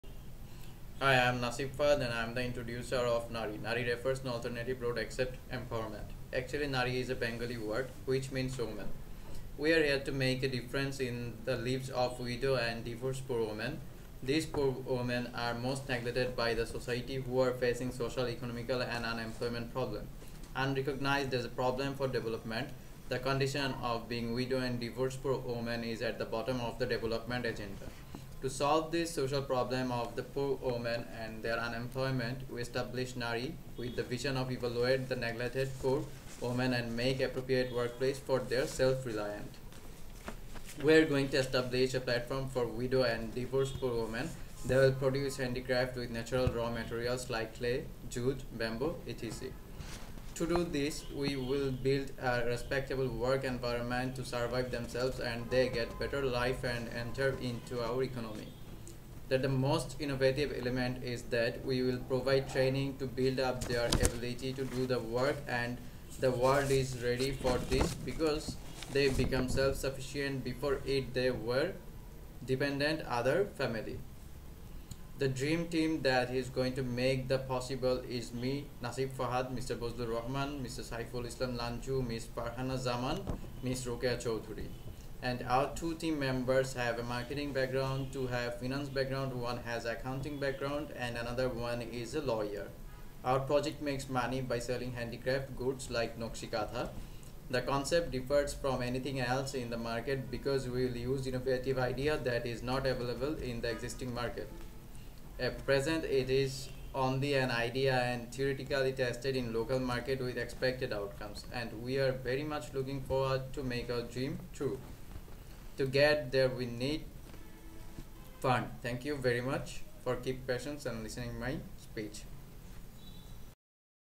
NAREE (No Alternative Road Except Empowerment)
NAREE is a Bengali word, which means "Women". Divorced and widow women are becoming a social crisis for our country as we don't have enough job facilities. We are going to establish a platform for them where they will get a respectable work environment to survive themselves and they enter to our economy. We made our business plan in an innovative way where we ensured financial sustainability. Those ladies will produce handicraft with natural raw materials like clay, Jute, Bamboo etc. so there is no question about environmental harm. Our goal is to create employment opportunity for them and they will do the social circle business.